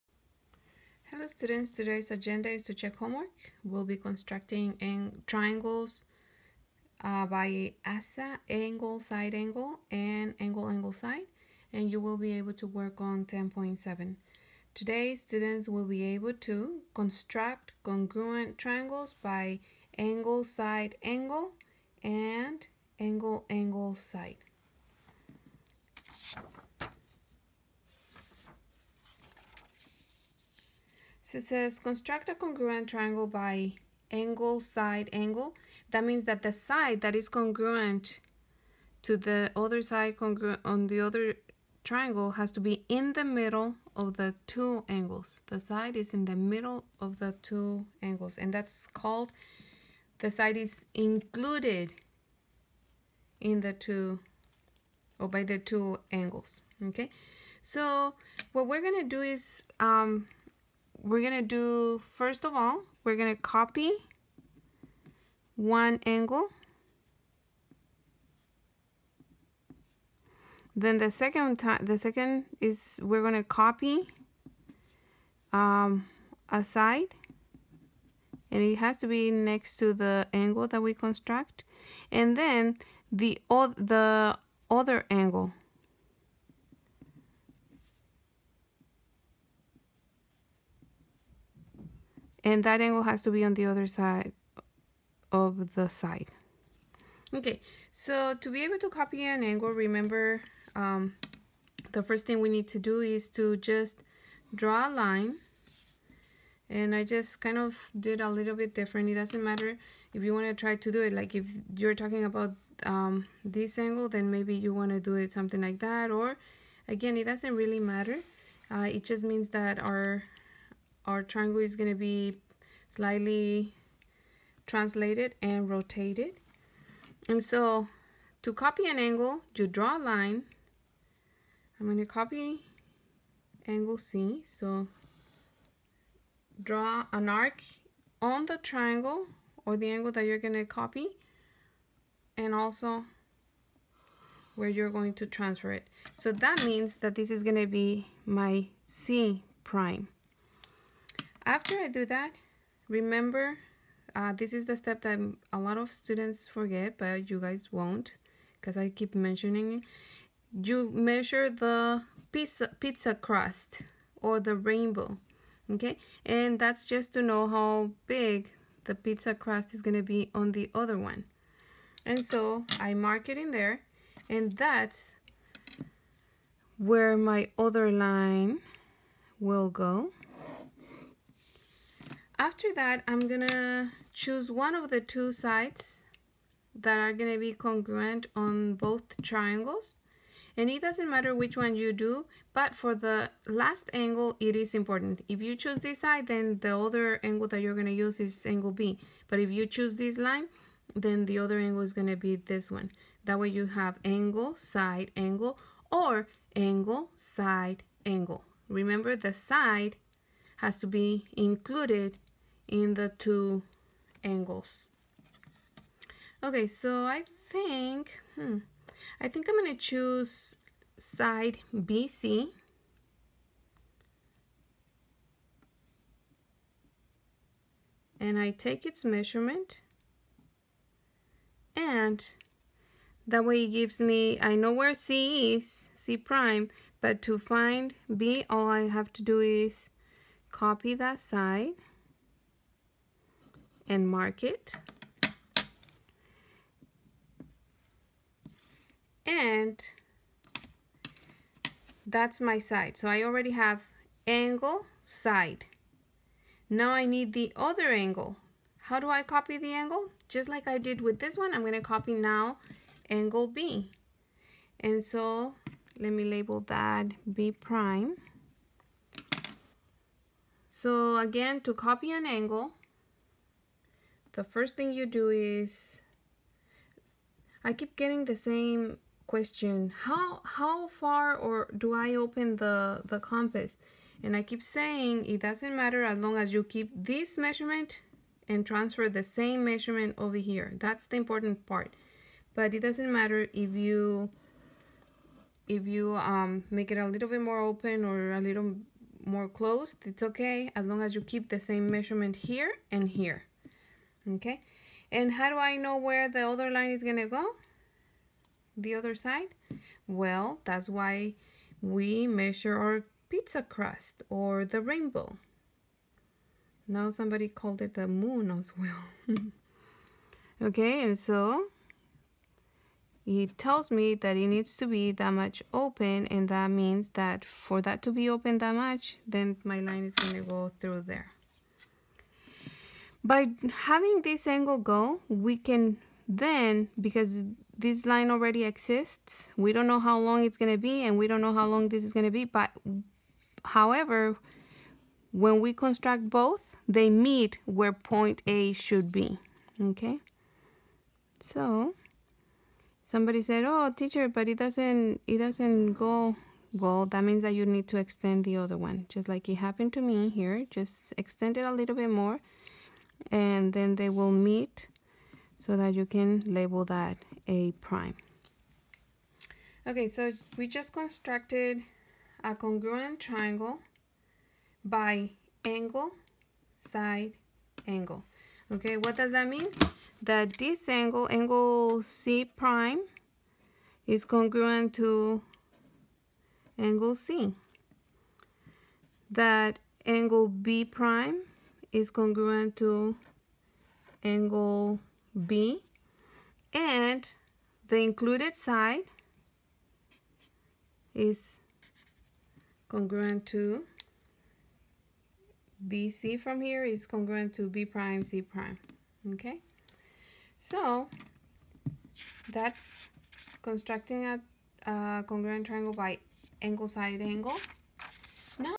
10.7a contructing by ASA
Students will be able to construct congruent triangles by ASA and AAS.